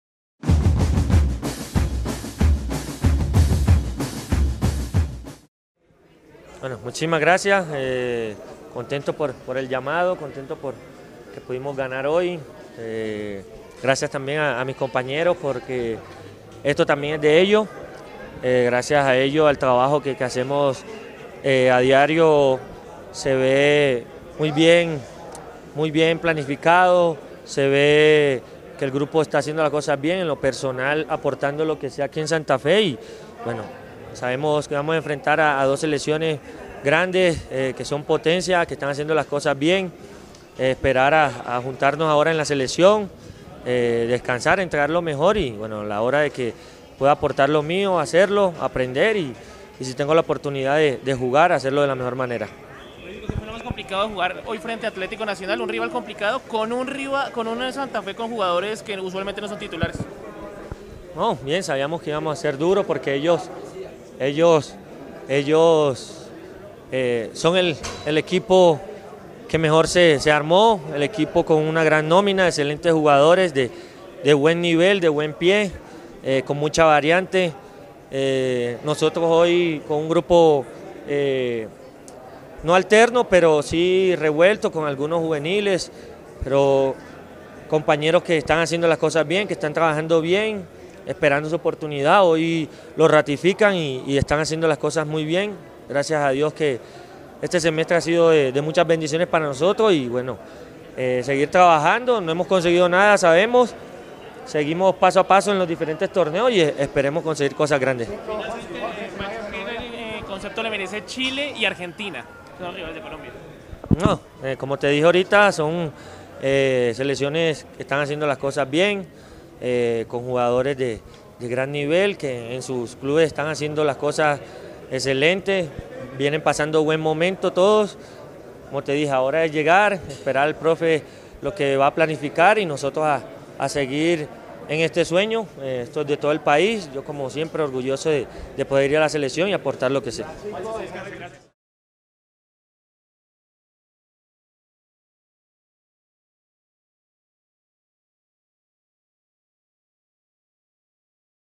[Reacciones Francisco Meza] Independiente Santa Fe (1) - Nacional (0) ::: Liga Aguila 2015-II
Reacciones de Francisco Meza luego de la victoria 1-0 de Independiente Santa Fe ante Atlético Nacional.